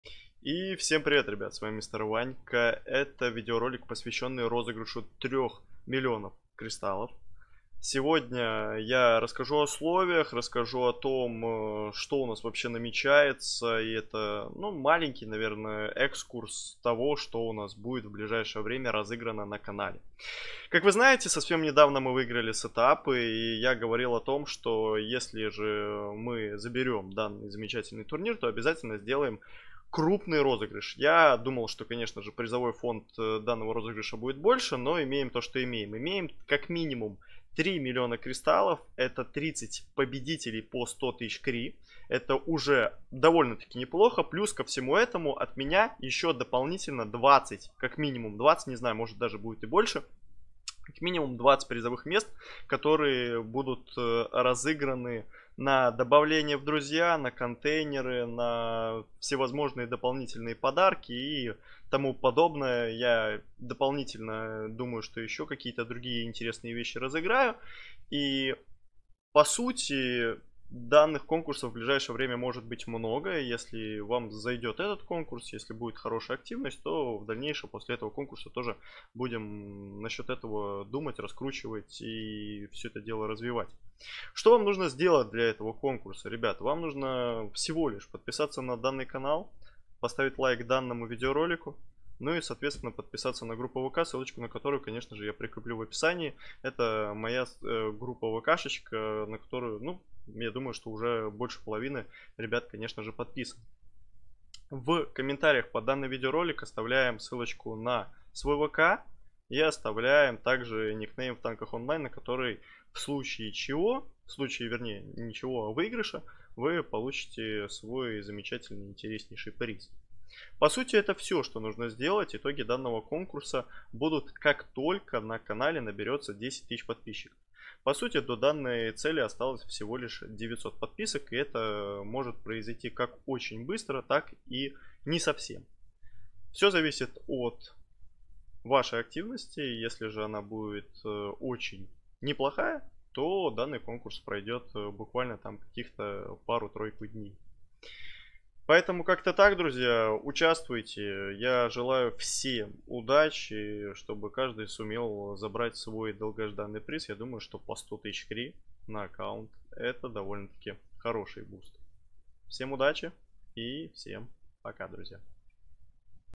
РОЗЫГРЫШ 3 000 000 КРИСТАЛЛОВ В ТАНКАХ ОНЛАЙН
Условия конкурса:
4.В комментариях указать ссылку на свой вк ( не полную ссылка а только id, например id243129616), а также написать свой игровой никнейм в Танках Онлайн)
Желаю всем удачи и пускай рандом вас не подведёт!)